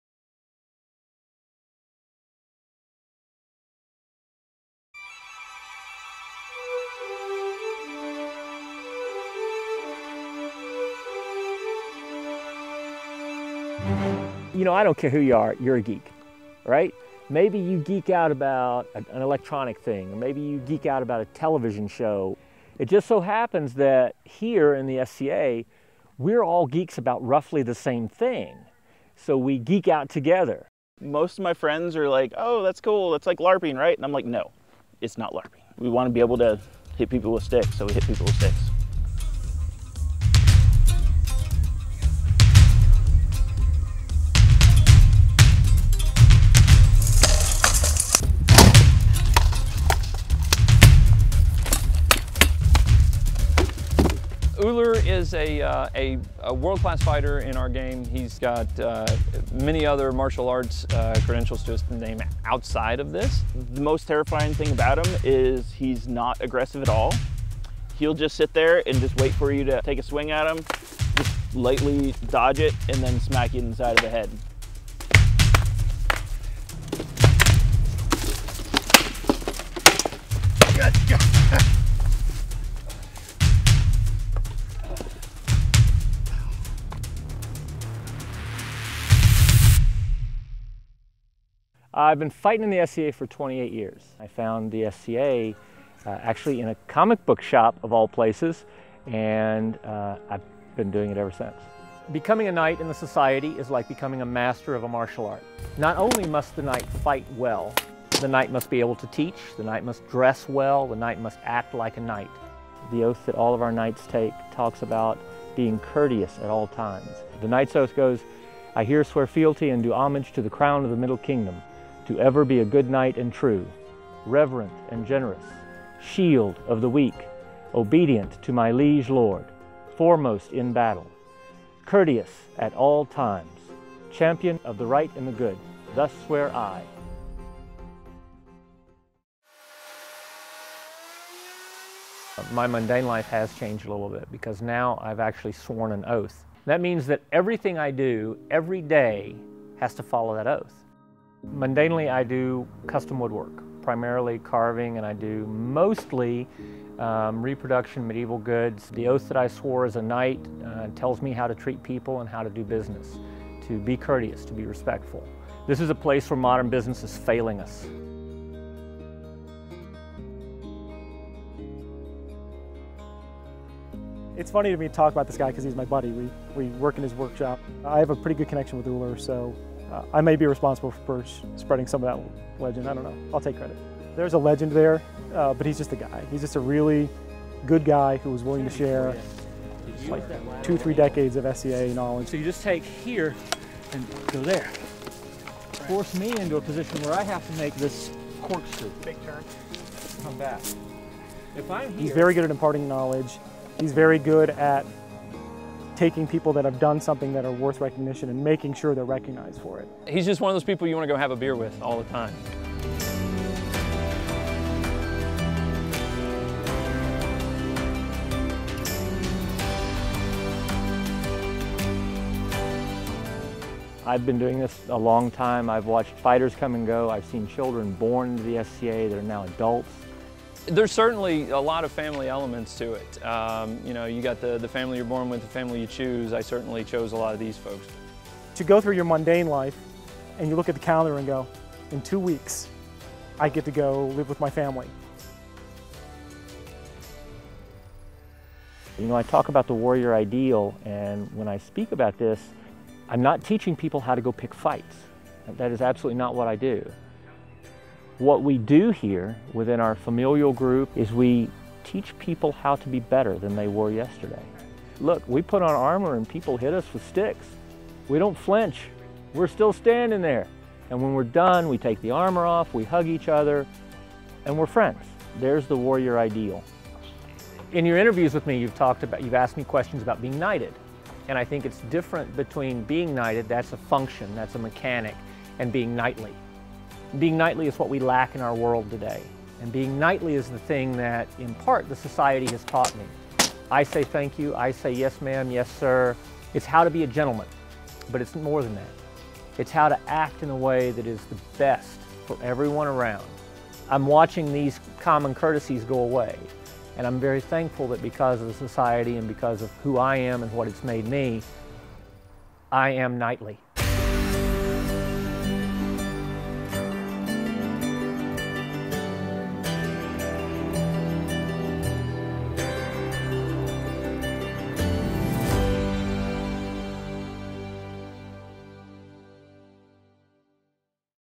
I Am Knightly
The family that fights together, stays together. At least, that’s what the members of the Society for Creative Anachronism would say. This group takes playing dress up to a whole different level, but behind the medieval dress, lies the heart of a true Gentleman. Enjoy "I Am Knightly."
Directed by Andrew King
Produced by Abby Witt
Camera and Editing by Landers Pannell